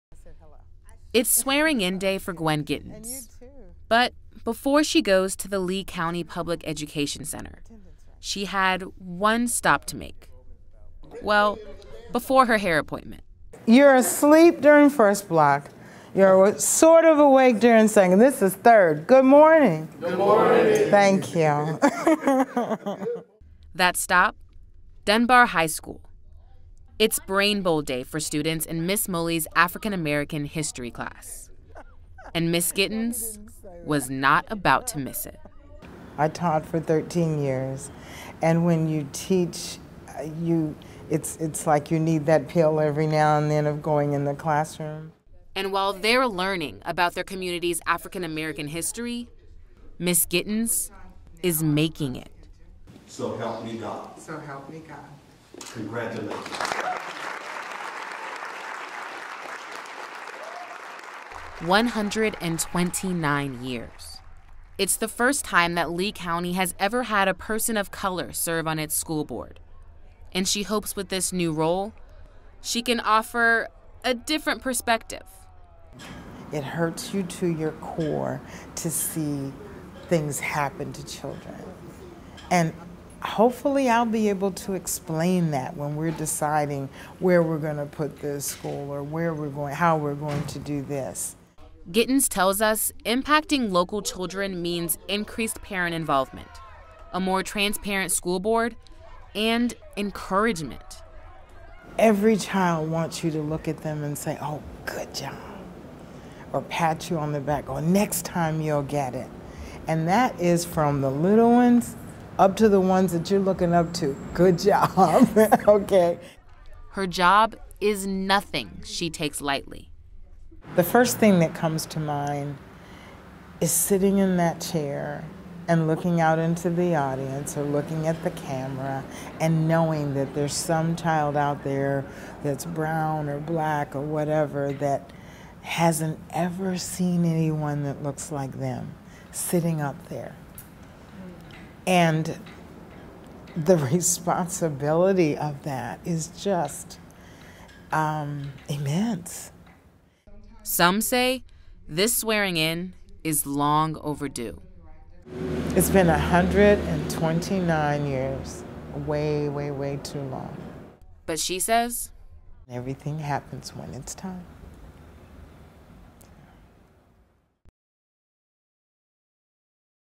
Gwyn Gittens: Plans to Make Lee County School Board More Transparent
In a time where the United States is struggles with racial divide, it's always inspiring to hear and see how minorities continue to break through glass ceilings.

“I’m unapologetically Black,” Gwyn shared during an interview with HelloSWFL.com.

“I bring empathy where there’s been sympathy because I’ve been through everything. However, I have succeeded getting to this point with the help of everyone in my community.”

After the pre-screened interview I was so inspired to assign Gwen's story to the perfect storyteller.

Enterprise Producer: Tamika Cody

Field Producer: Shari Armstrong

Storyteller: Jalyn Henderson